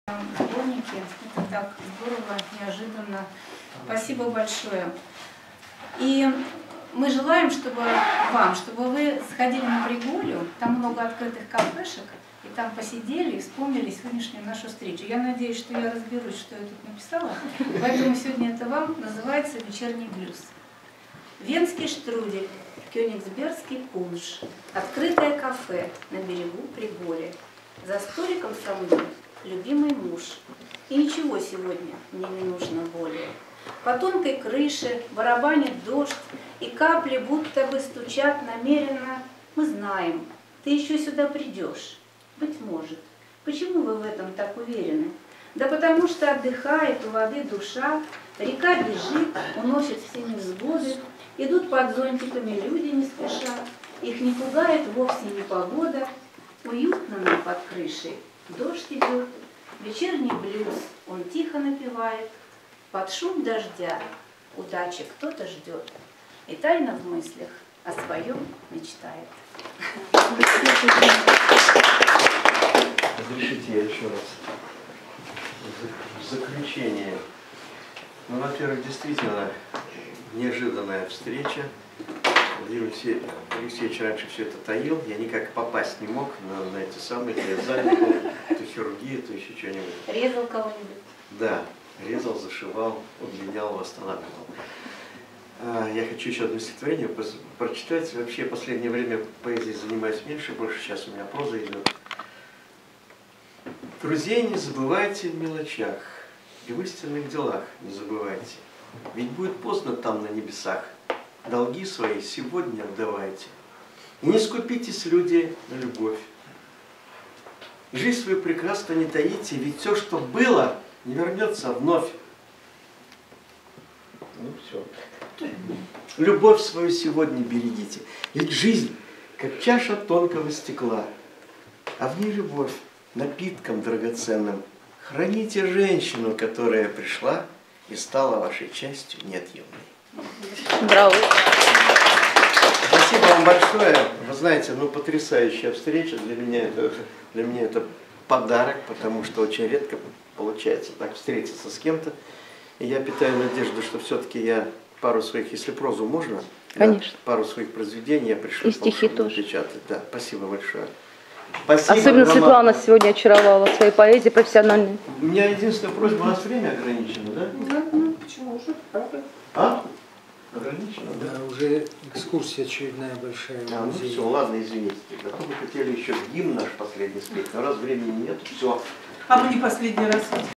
Татьяна Марьясова в библиотеке им. Пикуля в Балтийске
11 октября 2020 г.
г. Балтийск Калининградской области.
Члены ЛИТО "Остров Вдохновения" (председатель М.Б. Сорин) и участники Международного конкурса писателей, художников, бардов и журналистов "Калининград-янтарный берег" провели в военной библиотеке им. Пикуля совместное мероприятие, в рамках которого был презентован сборник произведений участников конкурса.